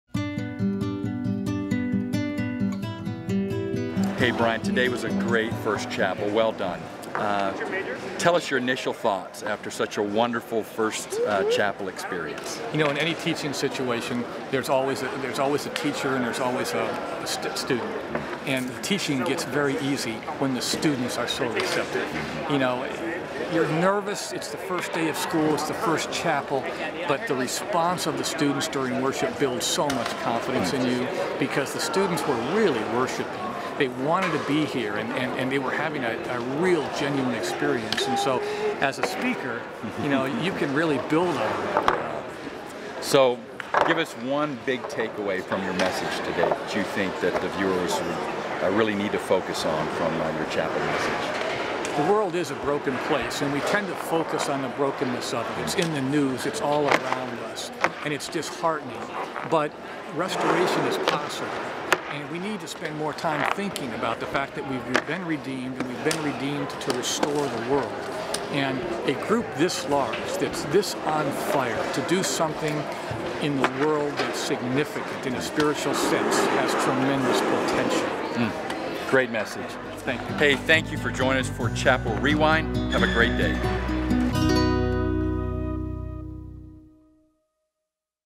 Chapel Rewind 8-26-14
Pastor Tim Griffin joins GCU President Brian Mueller to recap Monday's Chapel at the newly-renovated GCU Arena.